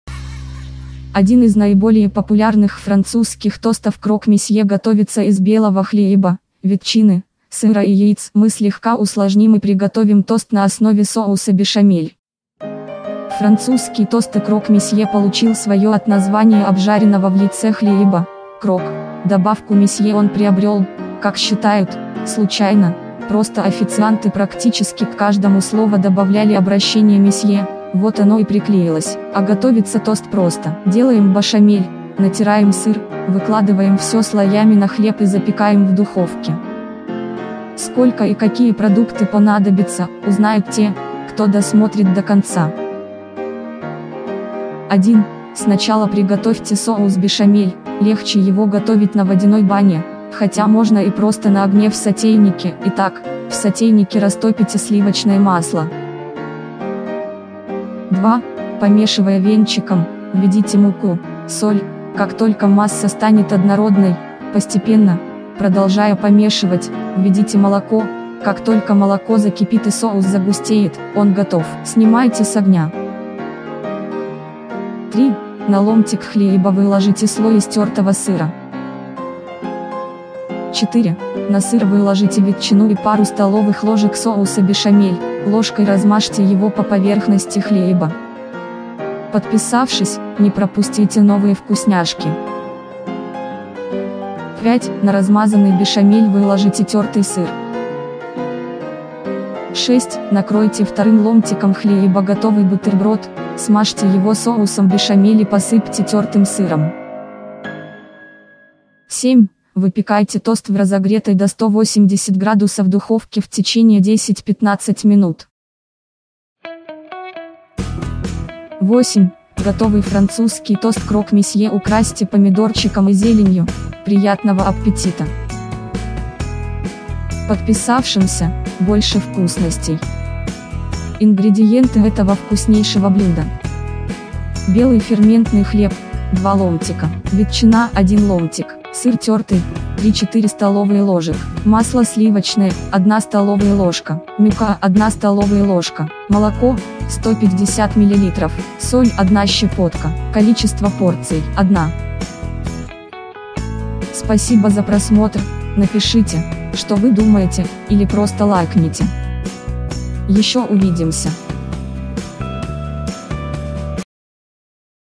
Французские тосты Крок-месье Рецепт, пошаговый, как приготовить, видеорецепт
Узнай идеальный рецепт! Один из наиболее популярных французских тостов Крок-месье готовится из белого хлеба, ветчины, сыра и яиц. Мы слегка усложним и приготовим тост на основе соуса бешамель.